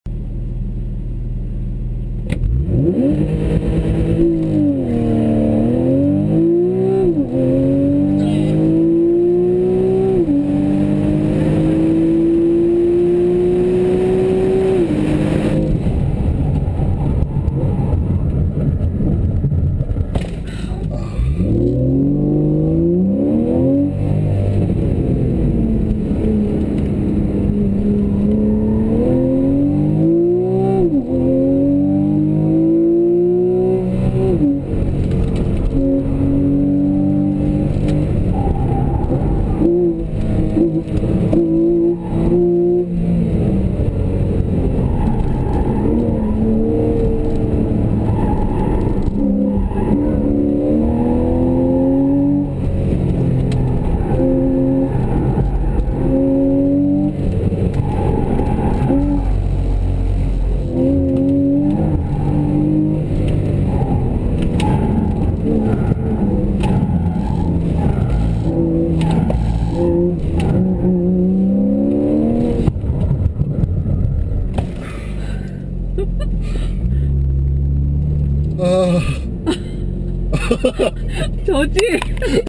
NEW 911 IMPRESSION
새로운 911 카레라S를 산타바바라의 공항에 만든 임시 서킷에서 타봤습니다.
박스터와 카레라를 합쳐서 반으로 자른 듯한 핸들링.
흥미로운 스로틀 스티어를 맛볼 수 있지만,
일부러 과격하게 조작해도 꽁무늬가 헛도는 일이 997보다 확실히 적습니다.

이러니 빠를 수밖에요.